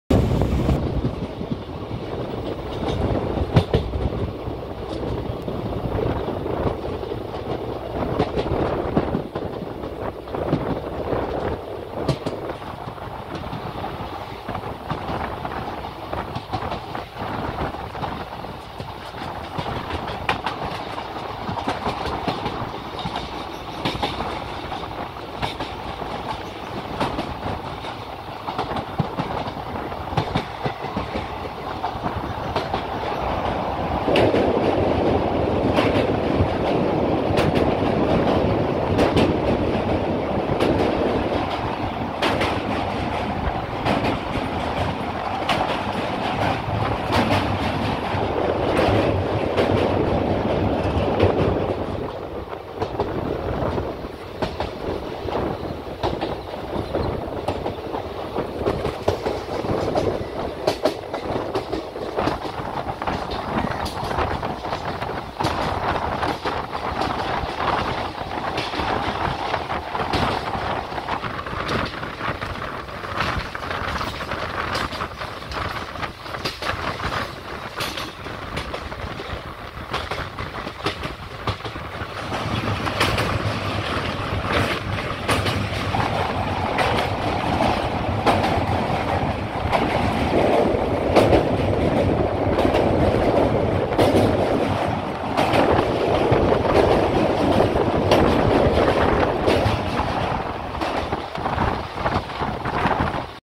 Mysuru-Tuticorin Express crossing Srirangapatna
16236 Mysuru-Tuticorin Express hauled by an electric engine crossing Srirangapatna at 18:30 hrs on 3rd March 2022.